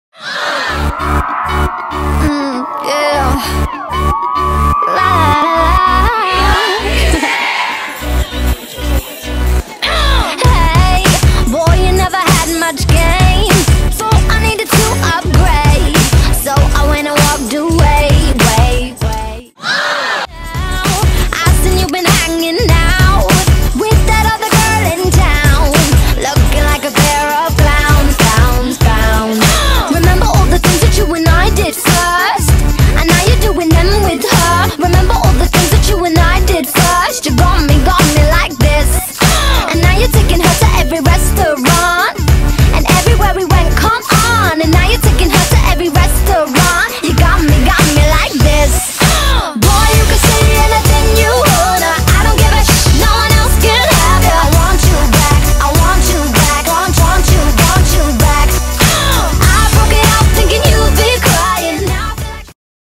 Cher Lloyd & Brats at Nashville's 107.5 The River
A cute little compilation of Cher and her fans from when she visited 107.5 The River in Nashville, TN!